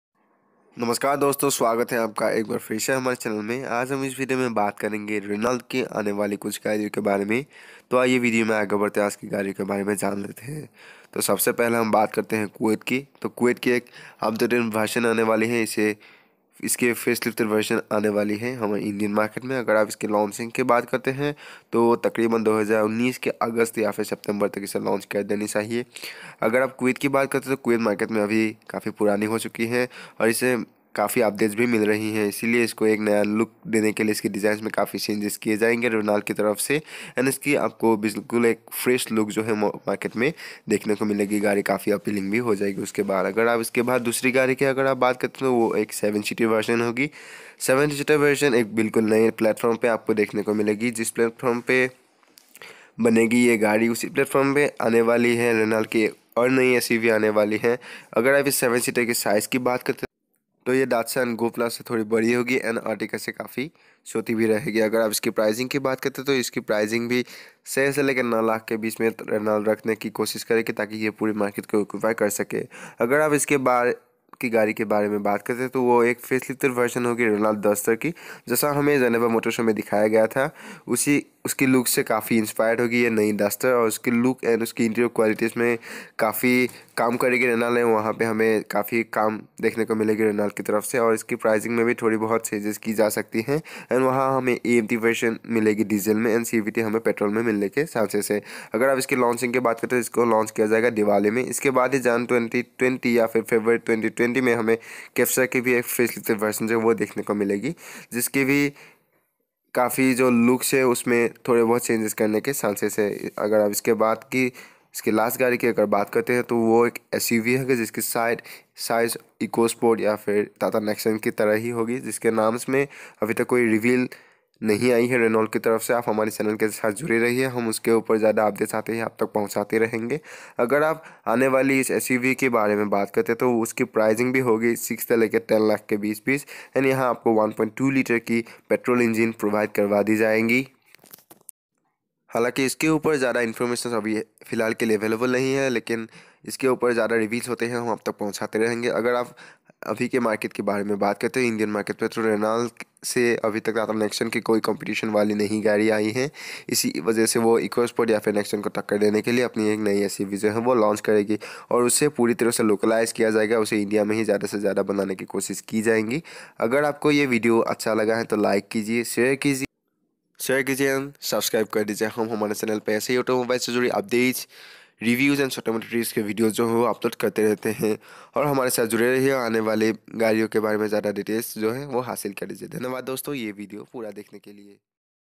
New generation KWID and upcpming new suv from renault
In this video we are informing you about 5 upcoming cars from Renault.
Please watch the complete video to know more.